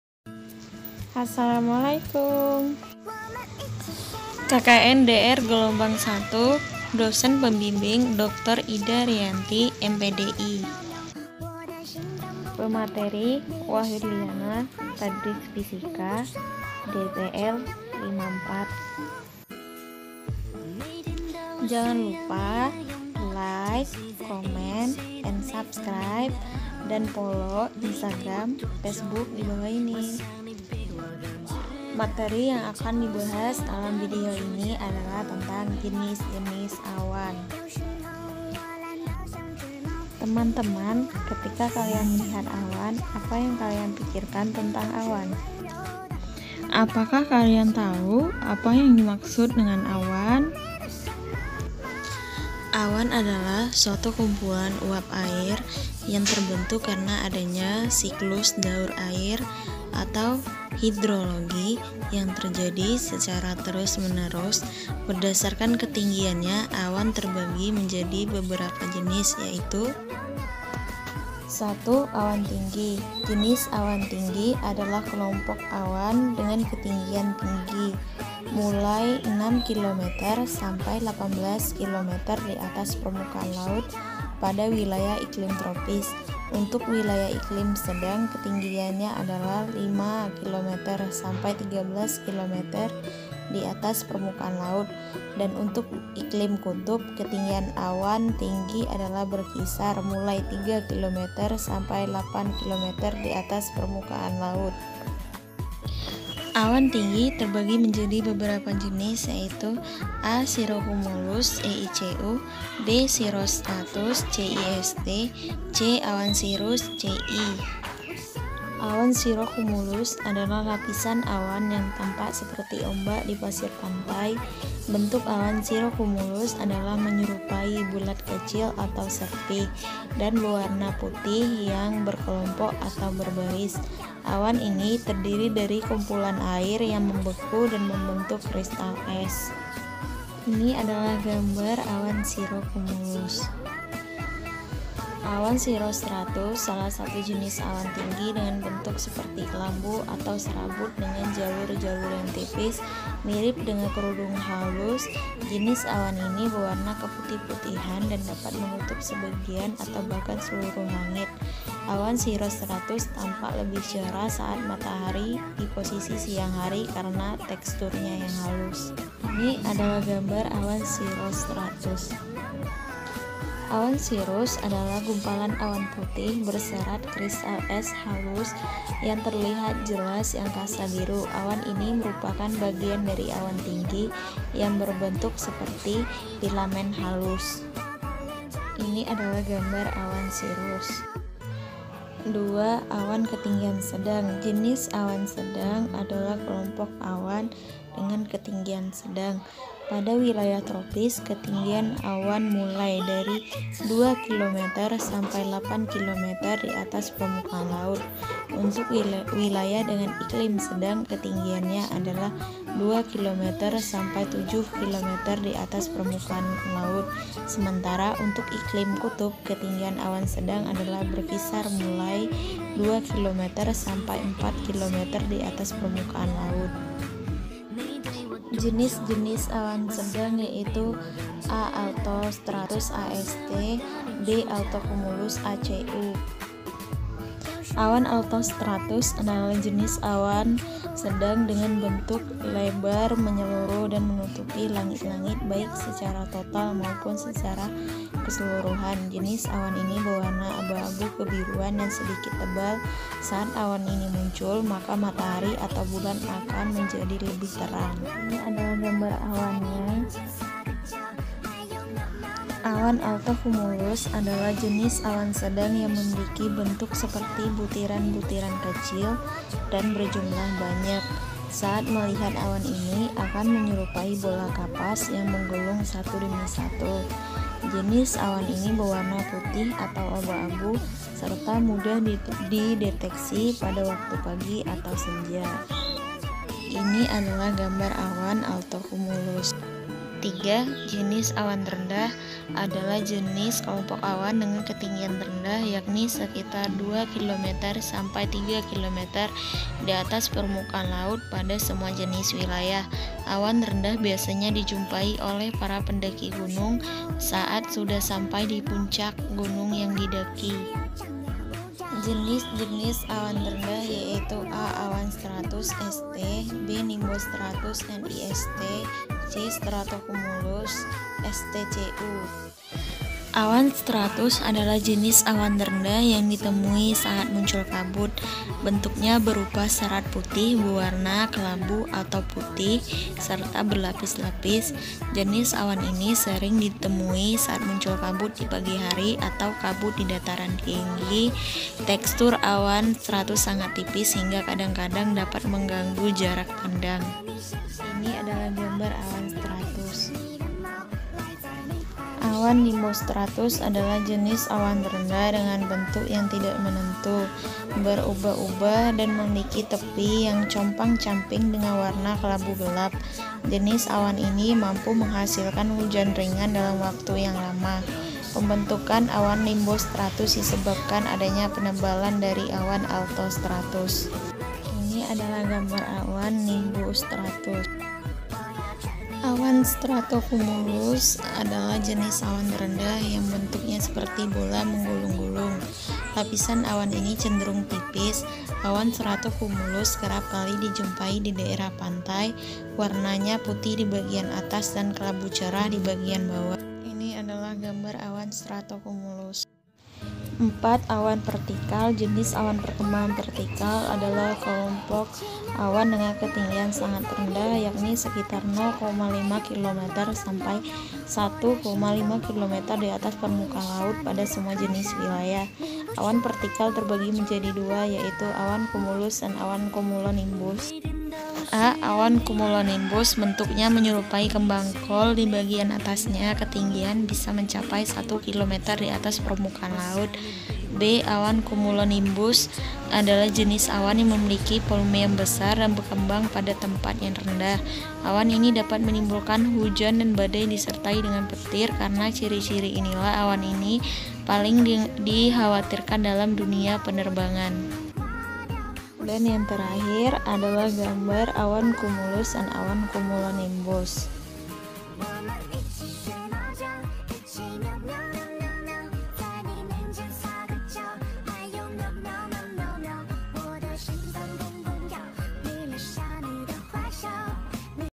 SEMUA AWAN ITU SAMA ??? YUKK KITA KENALI JENIS-JENIS AWAN INI!!!
Assalamualaikum semua.

video kelompok kami kali ini, kami akan membahas jenis-jenis awan dan ciri-cirinya agar teman-teman mengetahui bahwa awan itu tidak hanya indah di pandang mata, tetapi juga memiliki ciri-ciri yang berbeda

Saksikan yaa guysss..